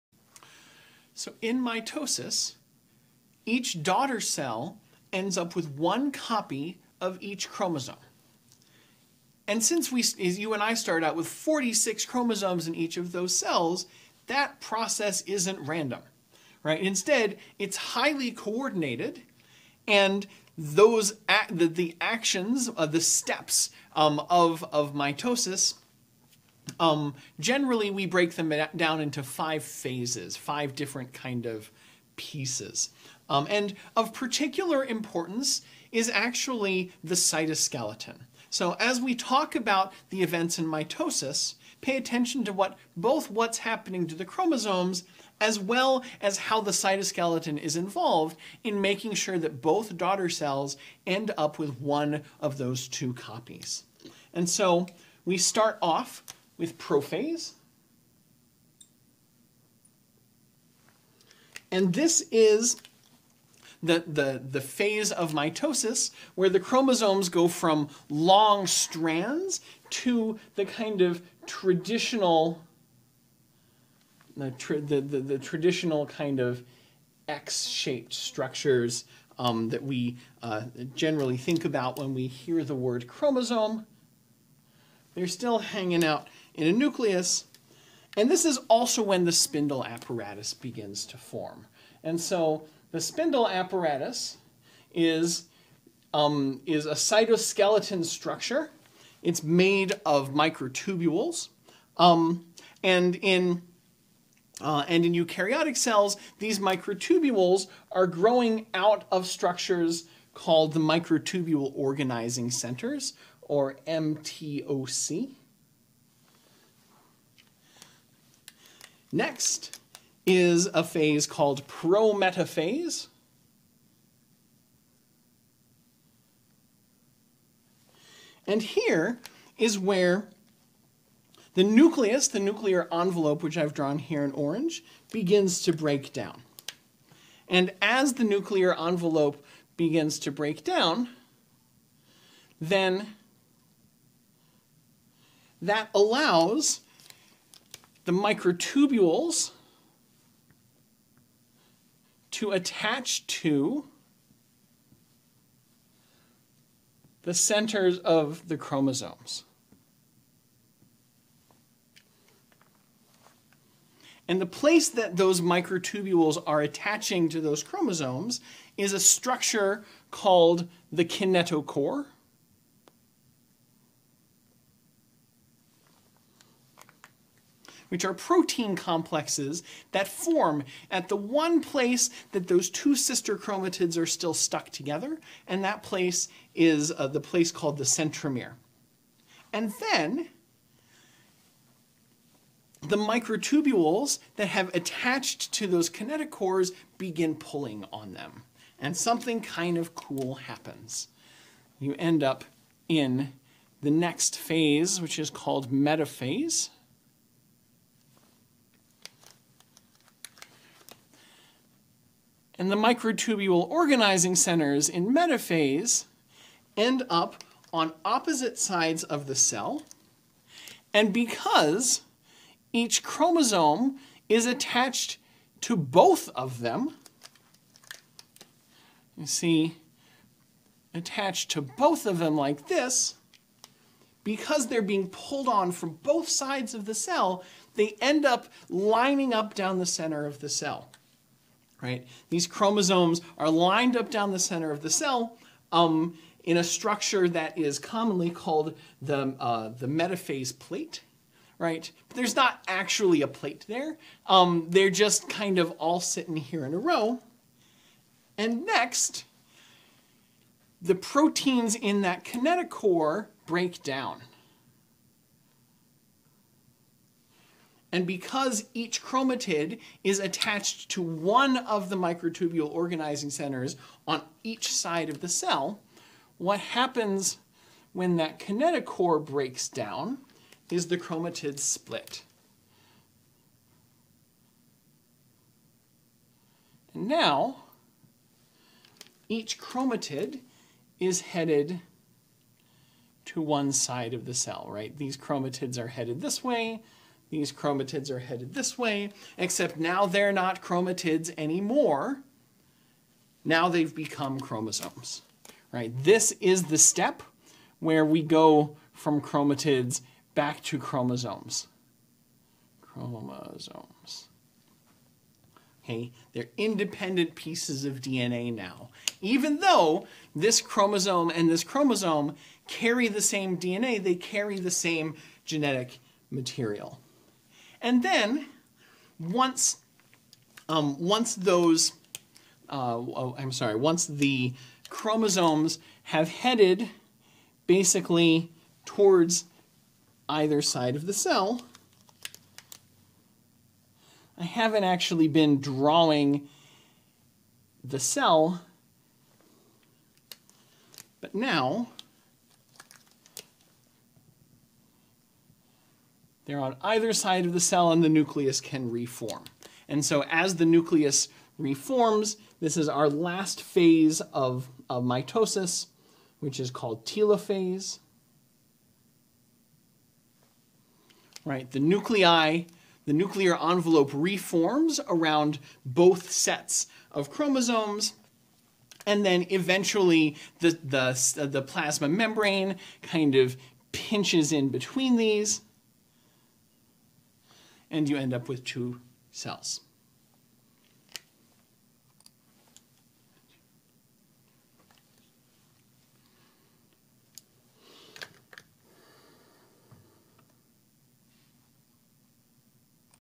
Mitosis
What is mitosis? What are the phases of mitosis? What happens to the chromosomes and how is the cytoskeleton involved? How do the steps of mitosis ensure that each daughter cell ends up with one copy of each chromosome?

This video lecture was recorded at the University of Wisconsin -- Stout in the fall of 2019. It accompanies my BIO 136 Molecular Cell Biology course, which was taught with Freeman's "Biological Science."